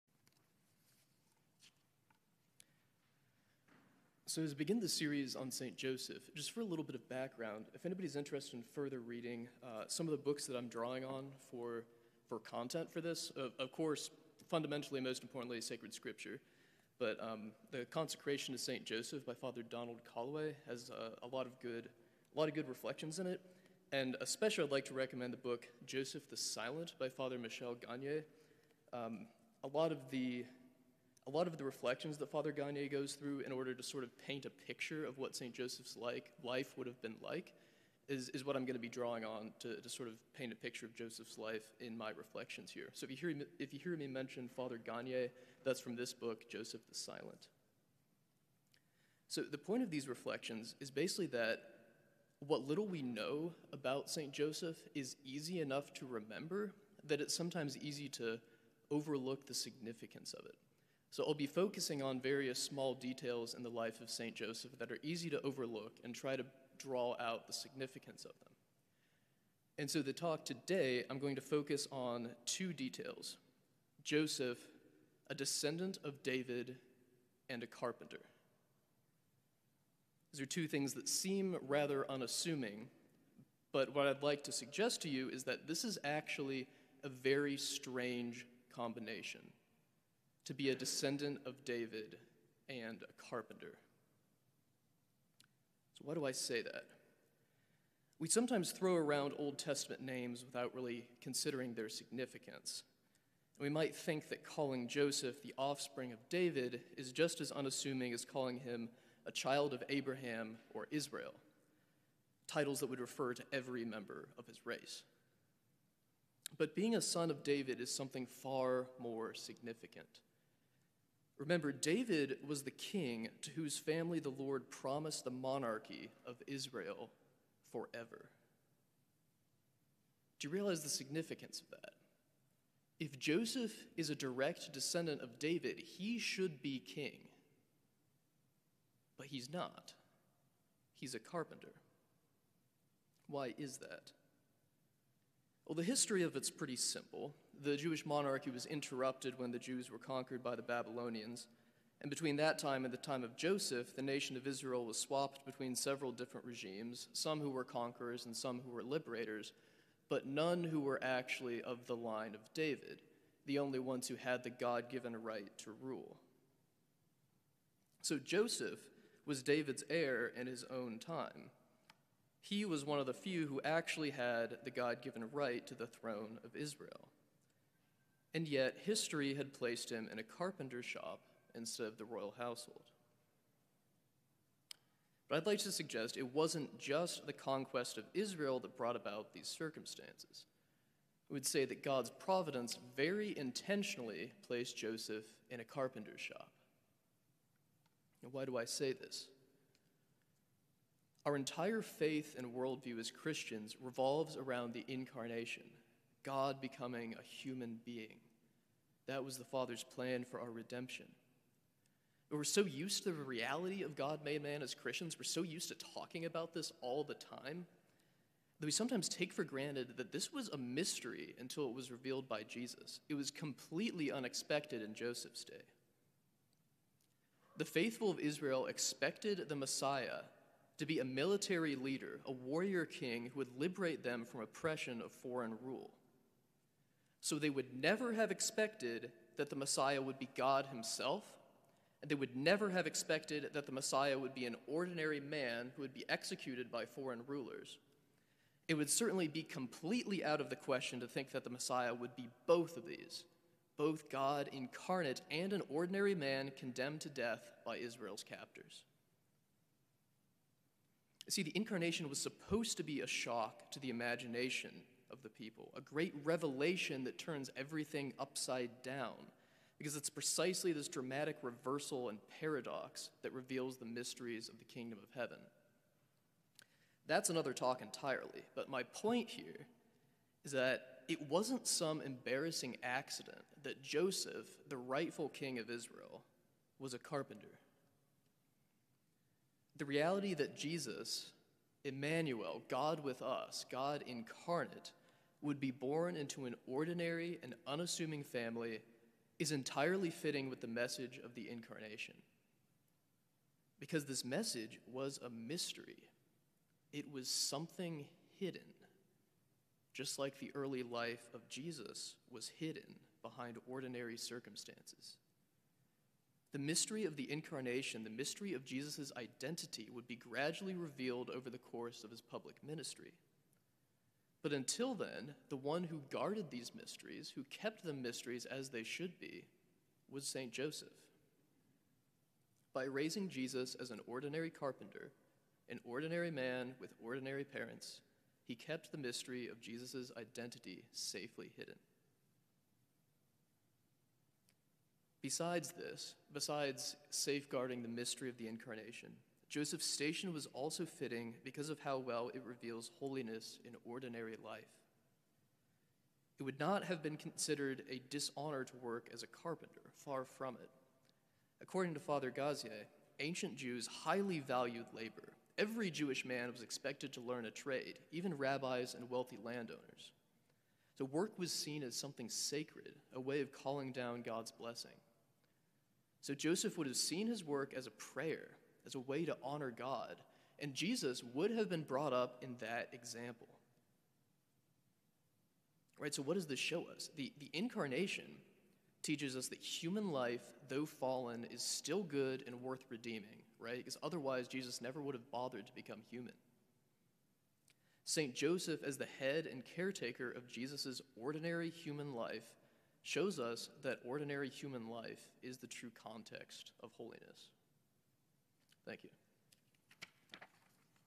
St. Joseph Reflection - February 20th
Our visiting seminarian, Andrew, shares about Joseph during this year of St. Joseph.